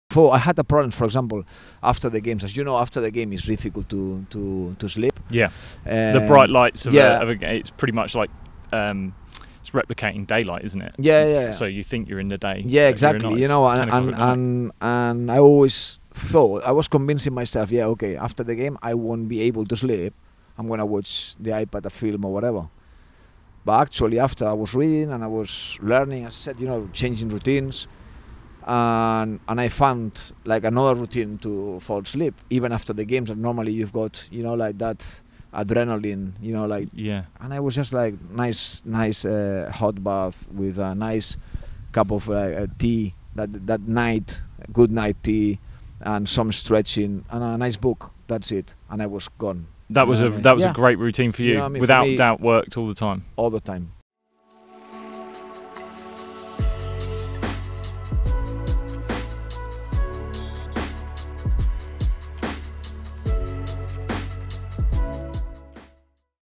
Bruno Saltor on His Post Game Routine after Football | Lewis Hatchett | Raising Your Game Podcast
Episode #4 Raising Your Game Podcast | Lewis Hatchett

Lewis Harchett interviews Bruno Saltor. Bruno Saltor talks about his post game routine after football.

⚽ Bruno Saltor - Spanish former footballer 🔵